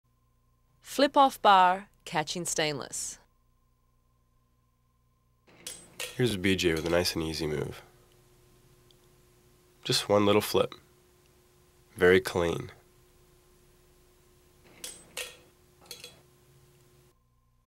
Easy Flair Moves #31 Strainer & Shaker Tin
Flip Strainer Off Bar, Catch In Shaker Tin.
Shaker Tins, or The Stainless, Are The Most Used Bar Object To Flair With. When You Master this small flair sequence, you can do it with any object.

Don't Underestimate the Simple Moves. A steady flow of many stylish, no risk moves that don't slow you down are your most valuable moves.
They get and keep people watching.

These Flair Moves Are Super Easy And Still Look Cool.
Over 240 Realistic, Practical Working Flair Moves In This Video Training Series. Use In Real Bar Situations.

Features: Billy Joe Checko